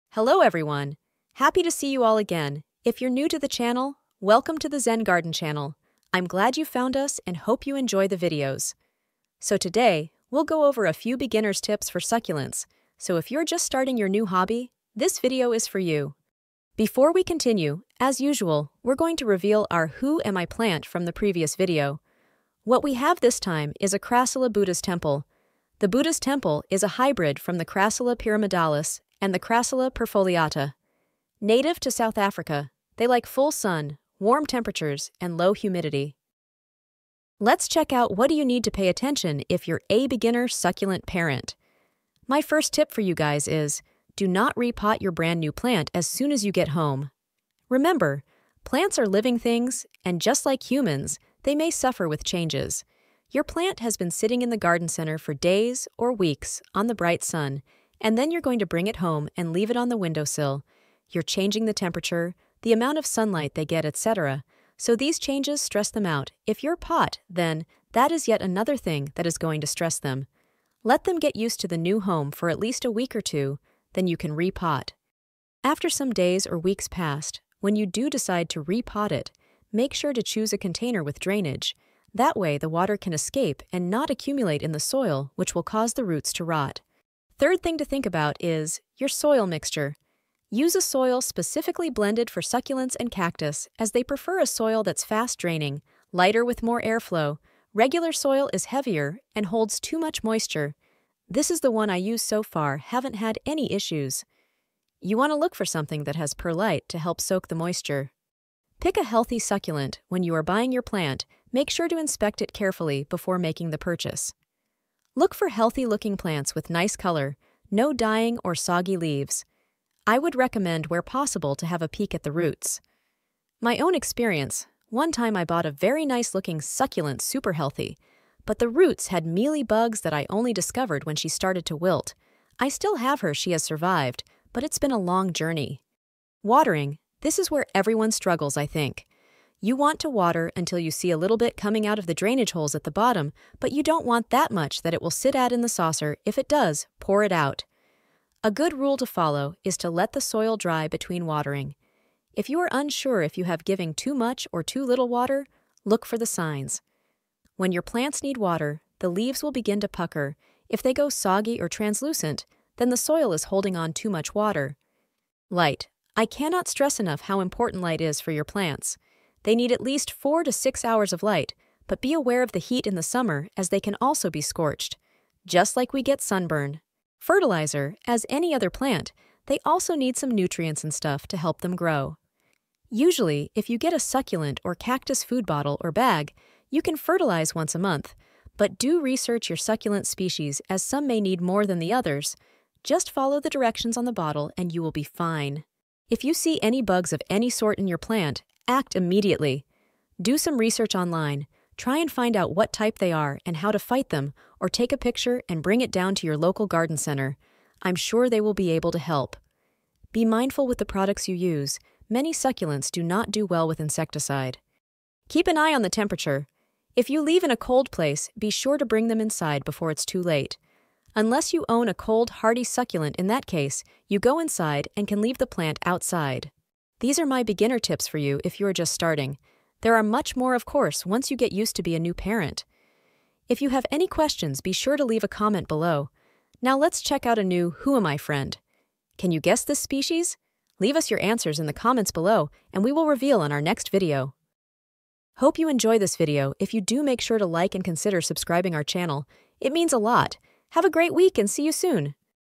🌿🌹Best tips for succulent & cacti beginners 😎🌱🎍
Who Am I? of this week is a crassula buddha's temple!
The Buddha's Temple is a hybrid from the Crassula pyramidalis and the Crassula perfoliata. Native to South Africa, they like full sun, warm temperatures, and low humidity.

Check out the one we are showing in this video! Tip: it's a very rare one that sells super expensive!

This video goes over a few of the things I have learn from my mistakes that you should do, or not!
When we're new to succulents its normal to hear a lot of different information so my advice here and any of my videos is from learning in my journey with succulents and other plants!
1. Wait 1 to 2 weeks before re potting your plant once you bring it home.
2.Choose a container with drainage holes
3.Use a succulent or cacti potting mix with perlite
4.Have a nice look at your plant before buying (avoid any stressed, dry or over watered plants, if you are just a beginner its likely you cannot save her, but by all means if you're brave then go for it! We can help!!)
5.Watering - Feel the soil to be sure its dried before the next watering
6.Light, be sure to give your plants at least 4 to 6 h light, if  you can give them more, better!
7.Fertilize them
8.Look for signs of pests so that you can act fast if you find any

I have killed a few and you will kill one as well, everyone does! That's how we learn!